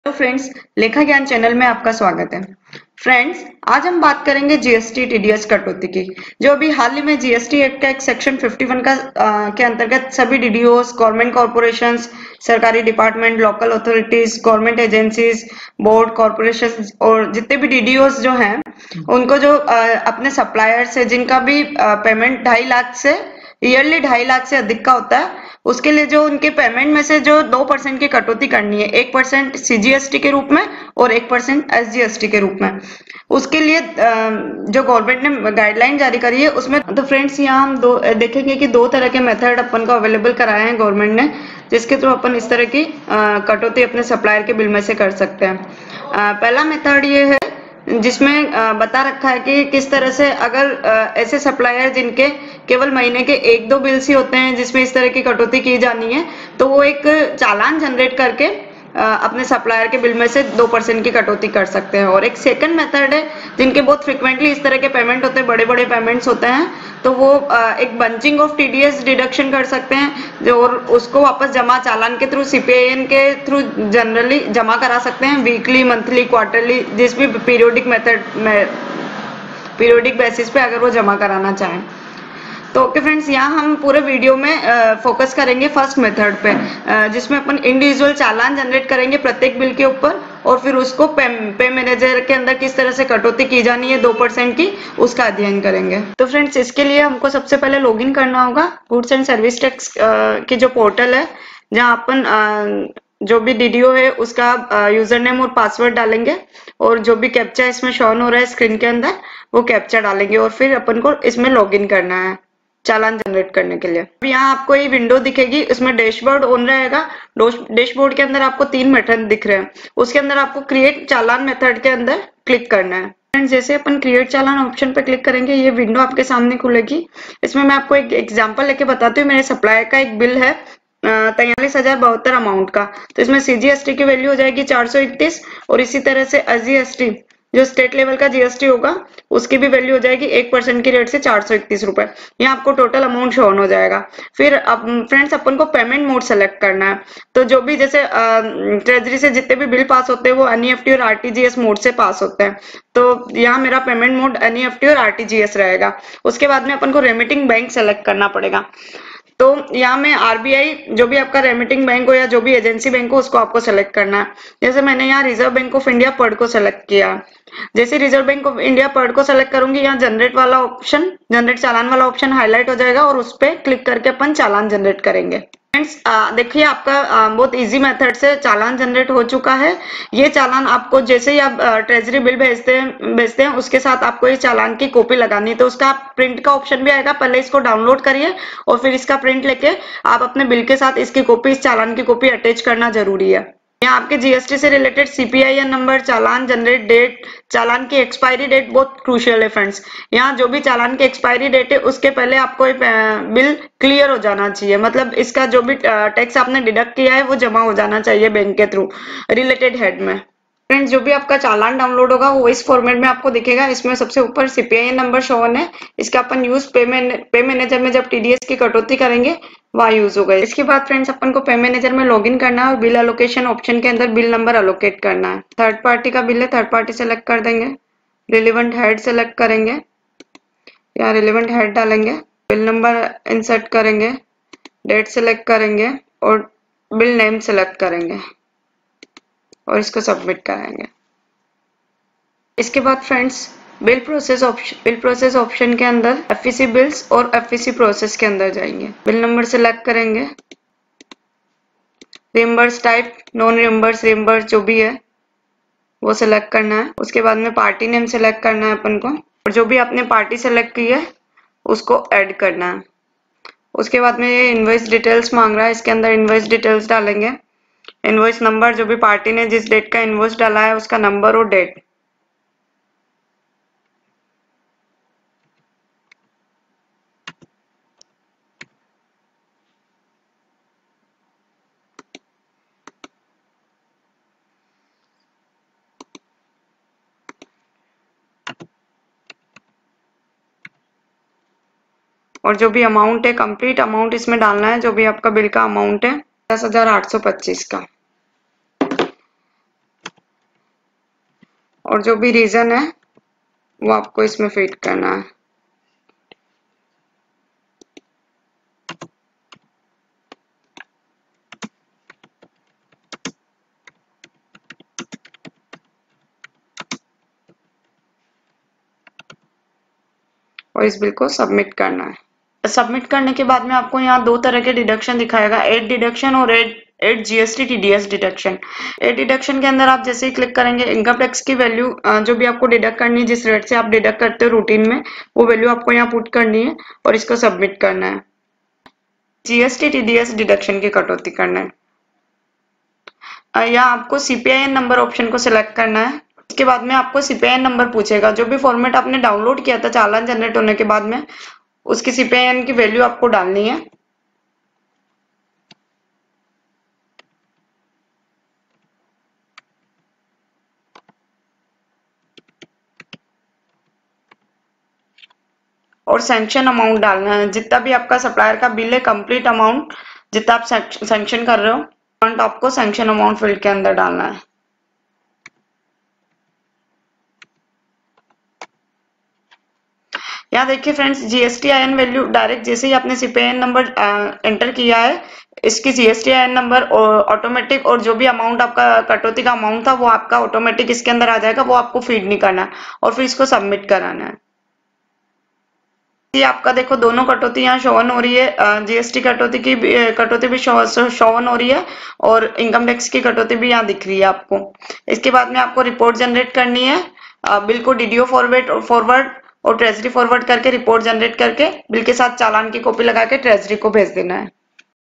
GST TDS bill on Paymanager / GST TDS by CPIN generation / Hindi / Option 1
video related to GST TDS deduction by DDO on paymanager. CPIN generation.